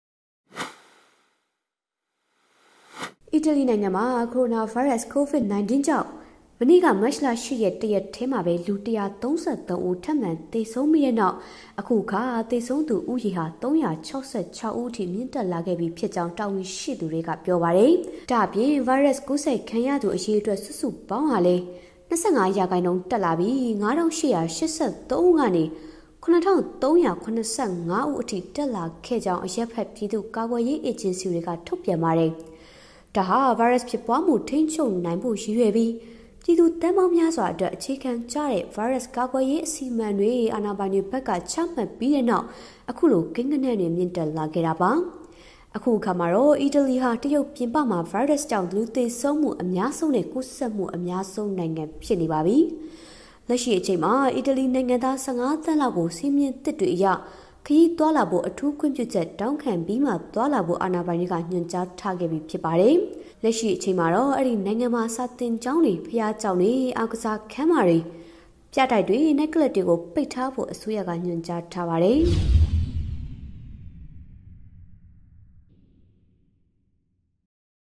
တရုတ်ပြင်ပမှာ COVID-19 ကူးစက်မှုနဲ့ သေဆုံးမှု အမြင့်ဆုံးနိုင်ငံ အီတလီ ဖြစ်လာ
လက်ရှိအချိန်မှာ အီတလီနိုင်ငံသား ၁၅ သန်းလောက်ကို စည်းမျဉ်းသစ်တွေအရ ခရီးသွားလာဖို့ အထူးခွင့်ပြုချက် တောင်းခံပြီးမှ သွားလာဖို့ အာဏာပိုင်တွေက ညွန်ကြားထားခဲ့ပြီ ဖြစ်ပါတယ်။

တင်ဆက် - လာဘ်ရတနာဗိုလ်